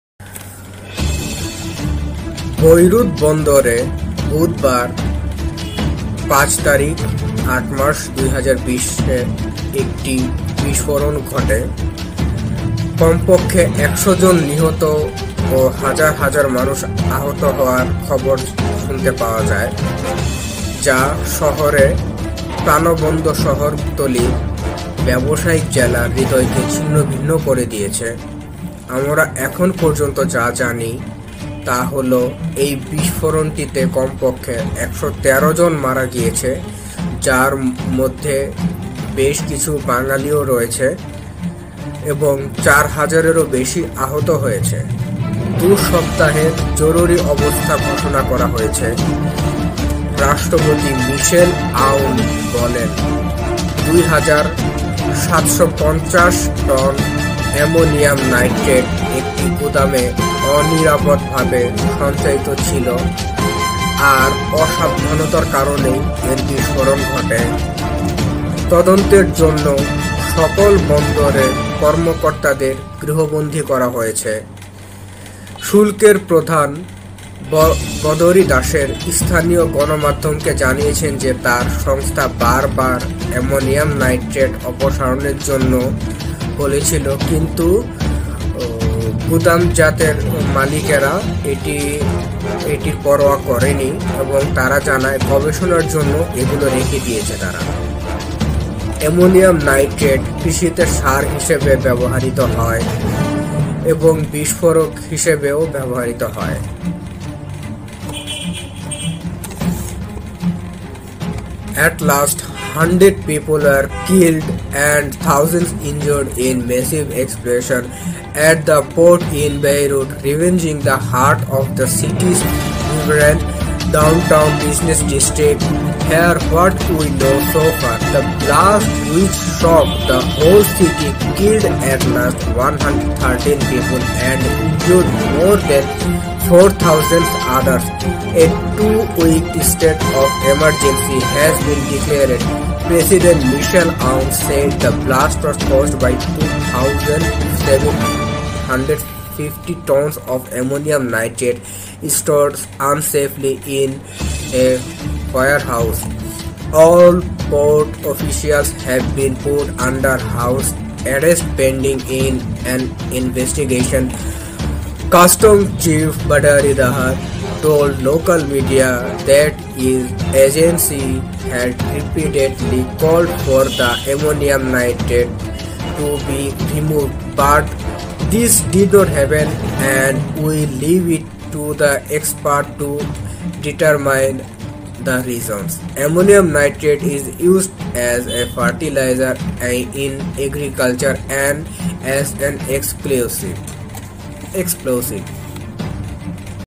লেবানন এর বৈরুত এ বিস্ফরণের আসল কারন জানুন।whats is Main reason of lebanon blast?lets watch.
Voice over Bangla and English